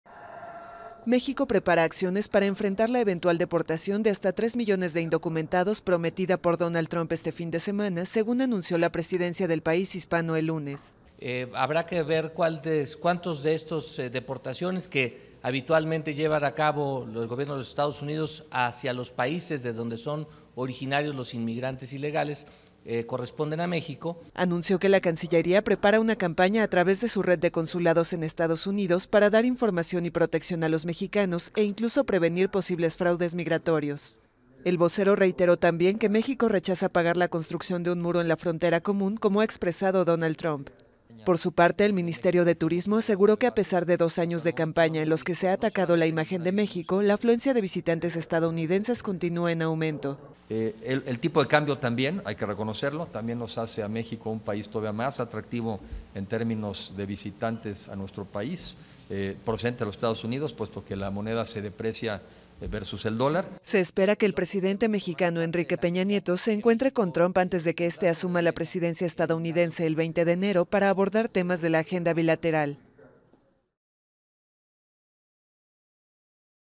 México se prepara para “contingencias” por gobierno de Trump
México anunció el jueves que está tomando acciones para “atender las posibles contingencias” que la presidencia de Trump podría tener tanto en el libre comercio como en los derechos de los migrantes mexicanos.